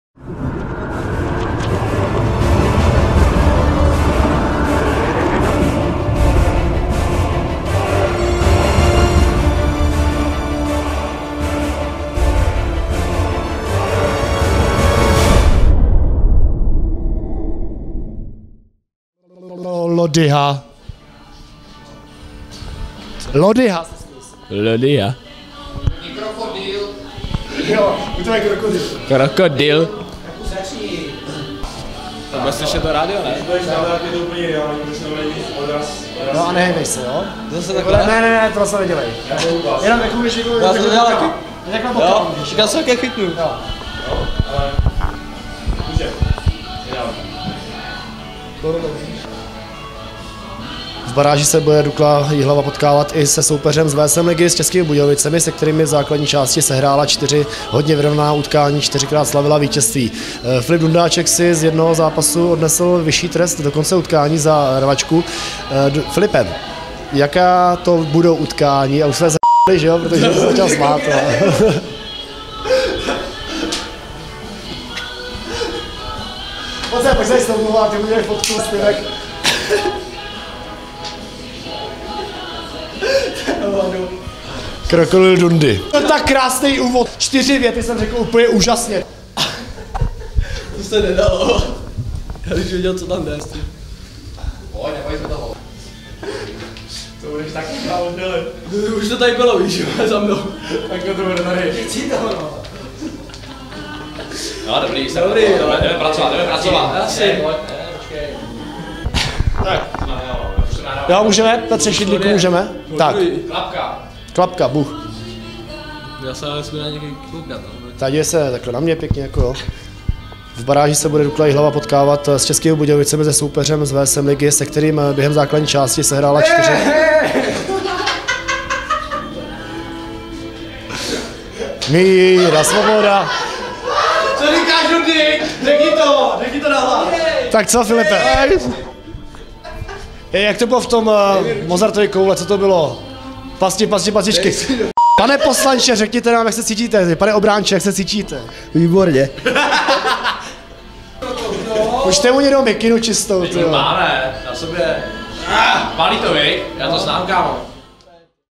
Baráž Dukly 2017: Jak vznikal rozhovor s Filipem Dundáčkem
Jihlavští hokejisté si nejen během celé sezony pochvalovali výbornou partu. Prakticky všichni se shodli, že to byl jeden ze základních stavebních kamenů úspěchu postupové sezony. Jejich slova nejsou lichá... Rozhovor s Filipem Dundáčkem k barážovému soupeři Dukly, Motoru České Budějovice, dal zabrat nejen redaktorům Zdeňku Drlovi a Tomáši Lysému, ale zejména zpovídanému obránci. Hlavními „vtipálky“ byli kapitán Tomáš Čachotský, obránce Petr Šidlík a gólman Miroslav Svoboda. A vysvětlení nápisu „Pavilon plazů – ZOO Jihlava“ je jednoduché. Krokodýl je živočich řadící se mezi plazy a jedna z přezdívek Filipa Dundáčka je „Dundy.“ Což je defacto podobné jako legendární americký film Krokodýl Dundee.